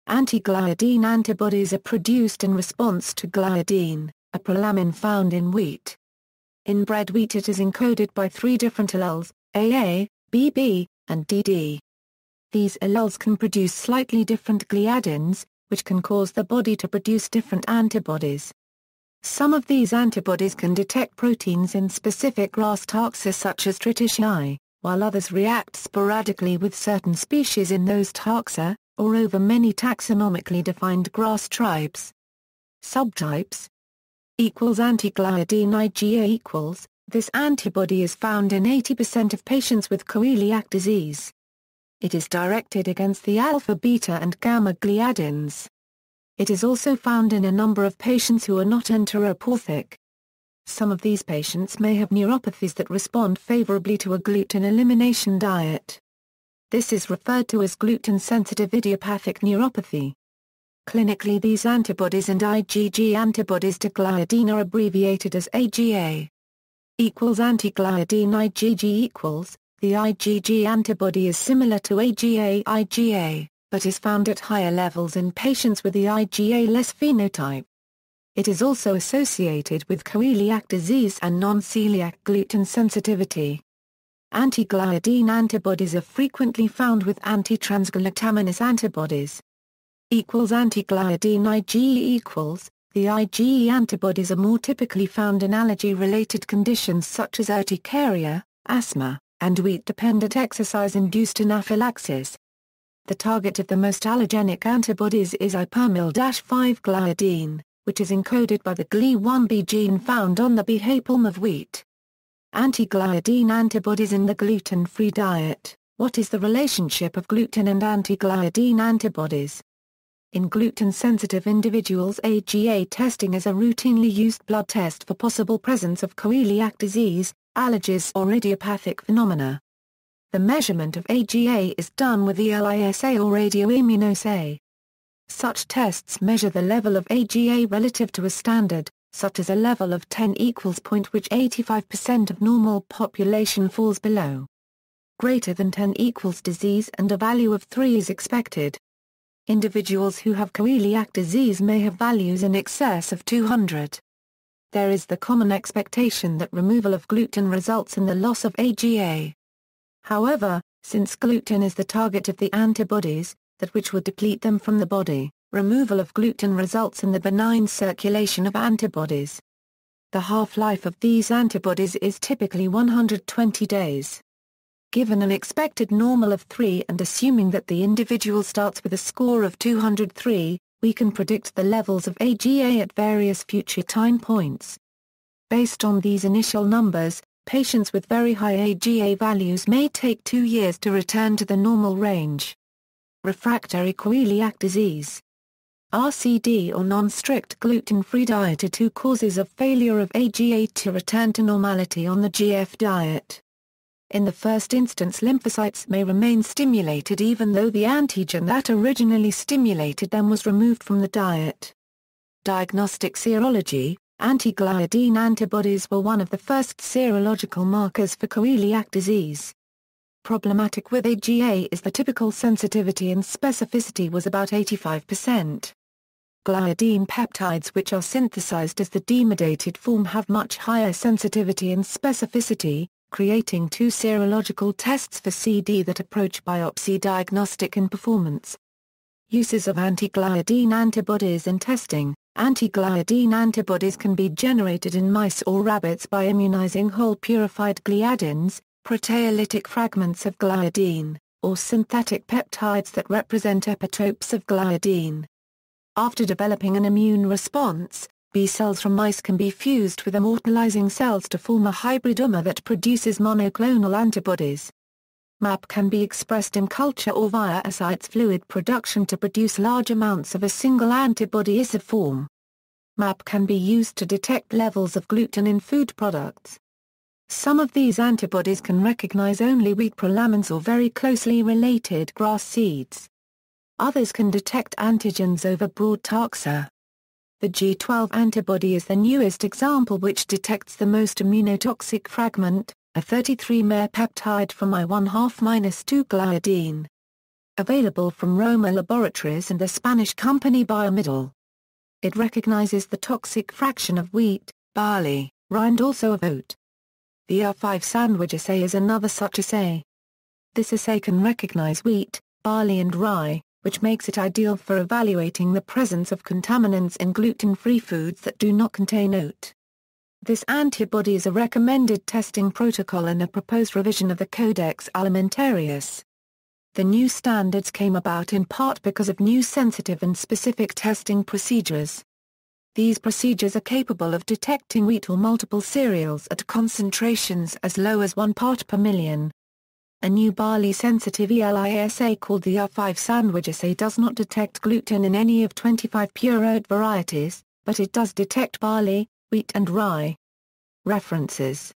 Anti-gliadin antibodies
Anti-gliadin antibodies are produced in response to gliadin, a prolamin found in wheat. In bread wheat it is encoded by three different alleles, AA, BB, and DD. These alleles can produce slightly different gliadins, which can cause the body to produce different antibodies. Some of these antibodies can detect proteins in specific grass taxa such as Triticeae, while others react sporadically with certain species in those taxa, or over many taxonomically defined grass tribes.